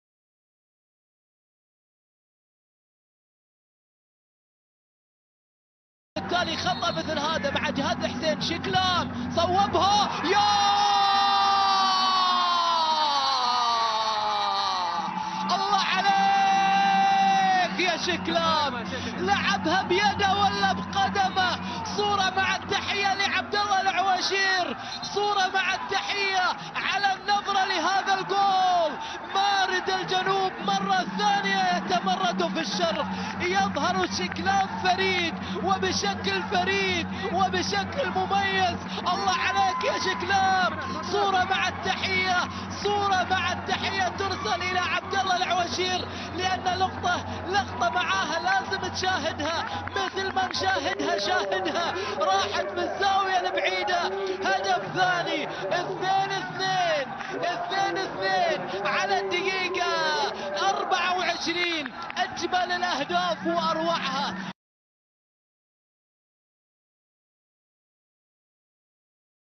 هدف الجزائري فريد شكلام على طريقة روبرتو كارلوس
Le but du joueur algérien CHEKLAM Farid comme la manière de Roberto Carlos sur coup franc dans un match VS Alfateh, qui s'est terminée par un match nul 2-2
هدف اللاعب الجزائري فريد شكلام على طريقة روبرتو كارلوس من ركلة حرة مباشره في مباراة الفتح ونجران والتي انتهت بالتعادل الايجابي 2 - 2
The goal of Algerian player Farid CHEKLAM like the way of Roberto Carlos free-kick in a game VS AlFateh , which ended in a draw 2-2